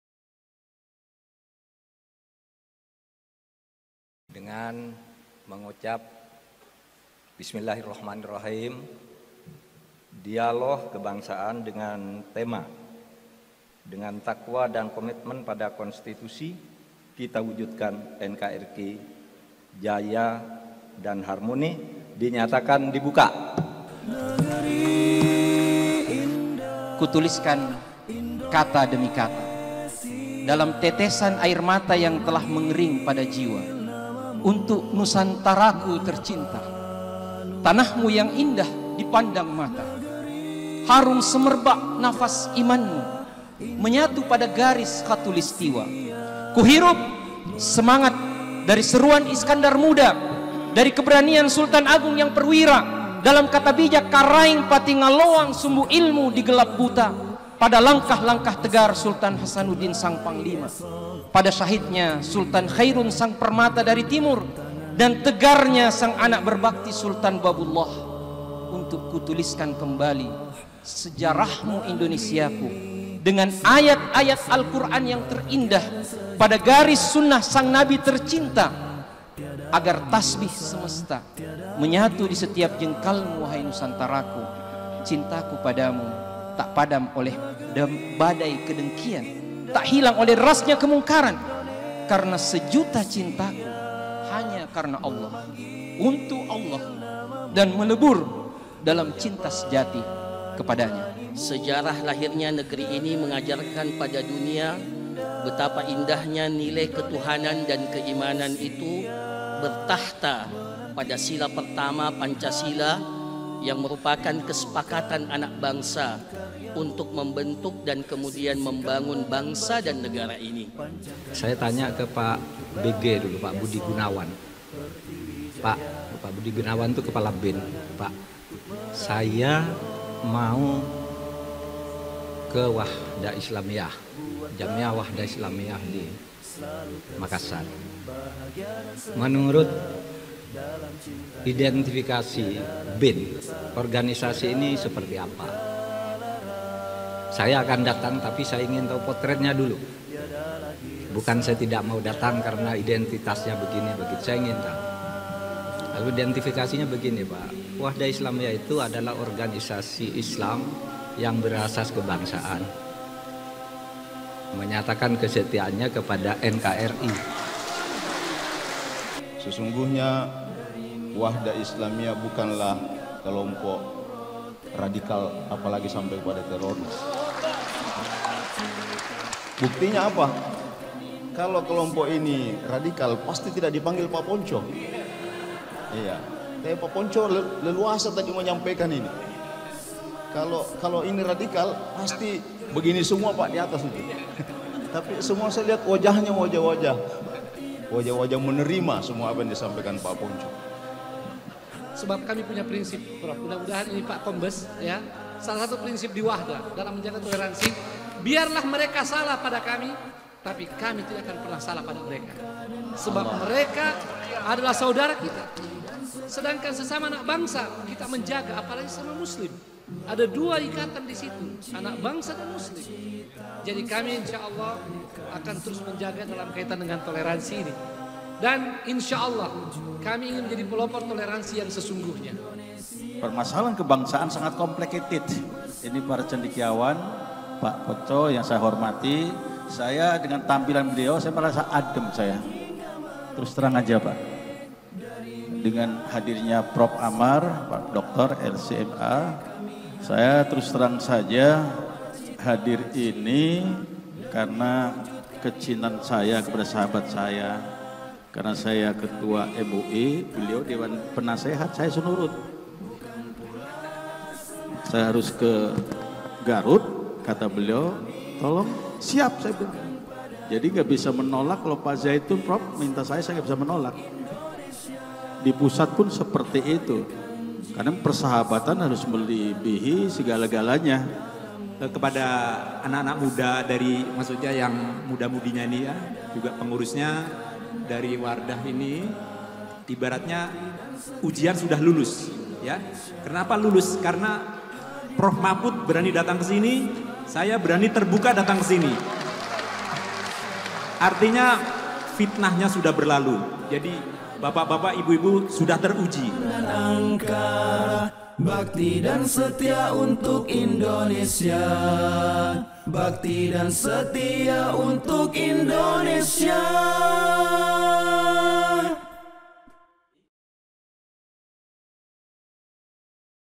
Highlight Dialog Kebangsaan - Event Nasional Agustus 2022 Wahdah Islamiyah
Cuplikan-cuplikan dalam highlight ini adalah penggalan-penggalan video Sambutan dan materi Narasumber dalam Dialog Kebangsaan yang digelar oleh Wahdah Islamiyah bertempat di Gedung Aula Aisyah Sekolah Tinggai Ilmu Islam dan Bahasa Arab (STIBA) Makassar